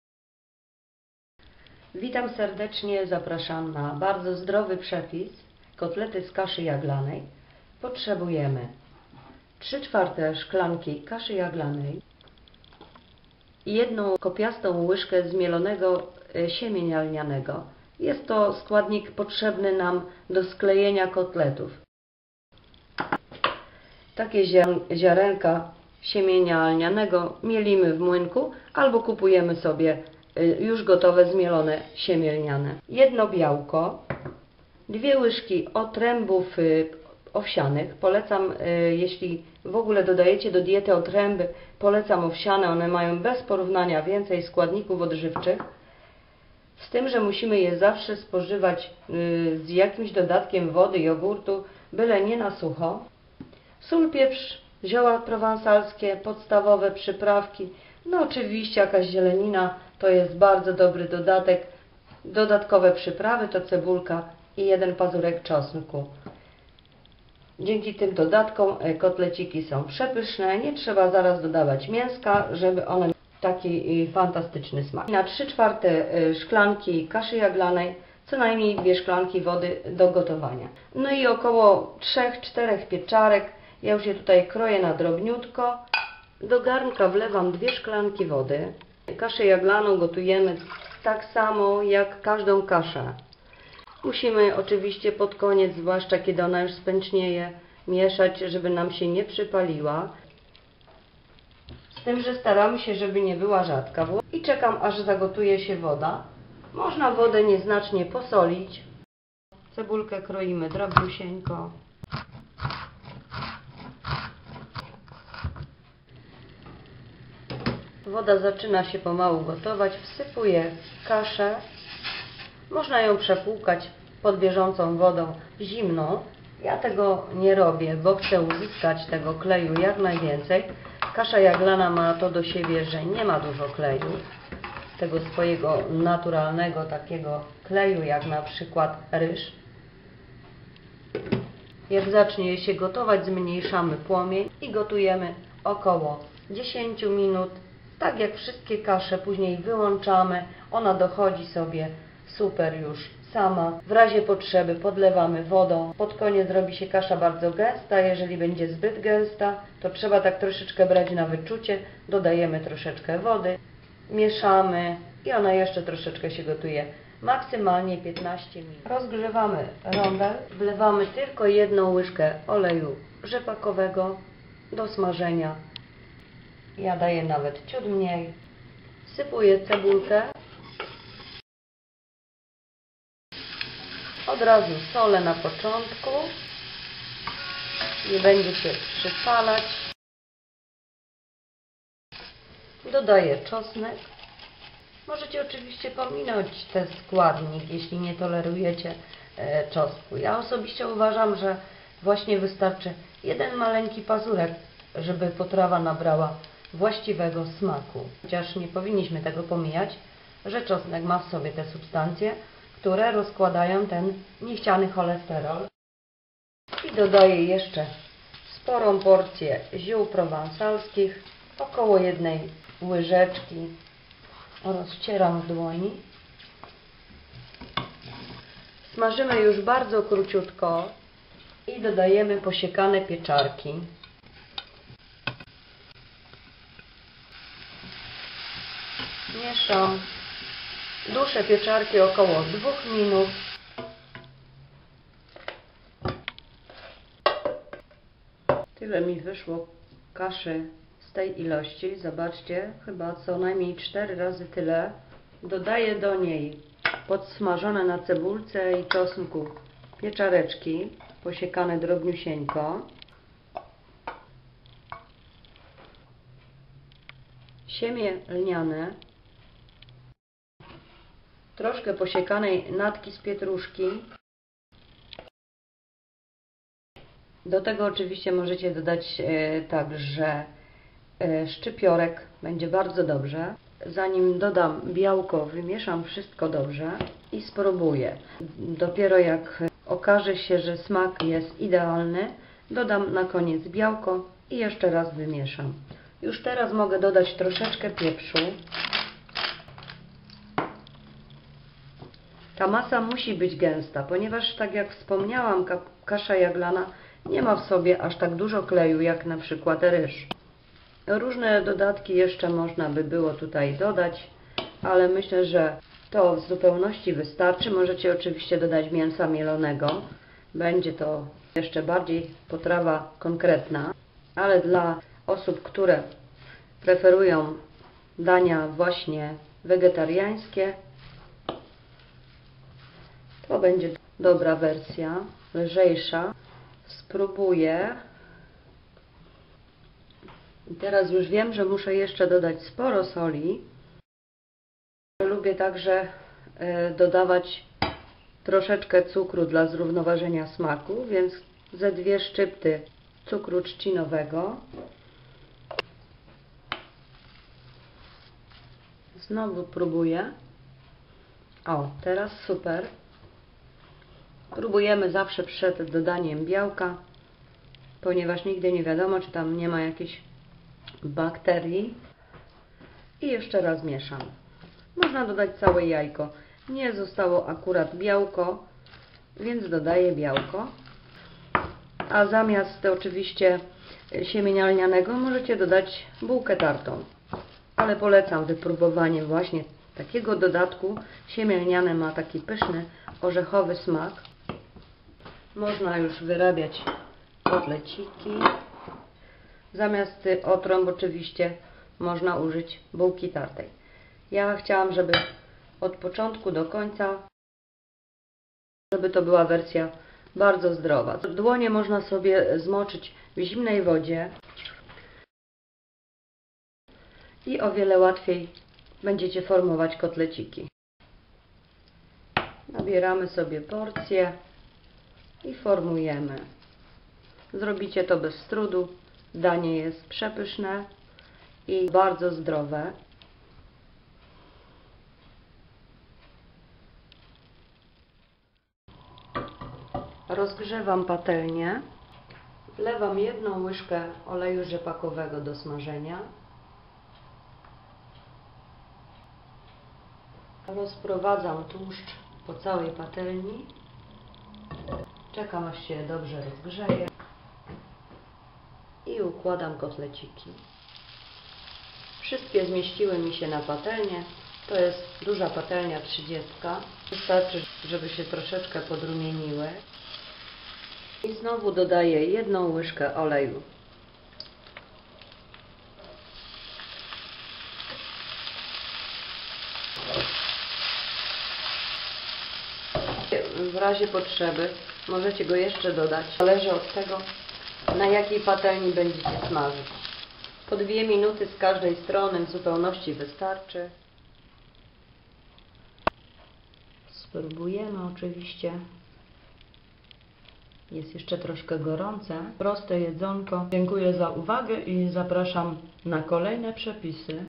Zdrowe kotlety z kaszy jaglanej
Witajcie
Przepisy z megazdrową kaszą jaglaną będą się pojawić na moim kanale systematycznie, bo jest obecna w mojej diecie od dawna. Dodaję ja do gołabków, pulpetów, do zup, czy zamiast ziemniaków.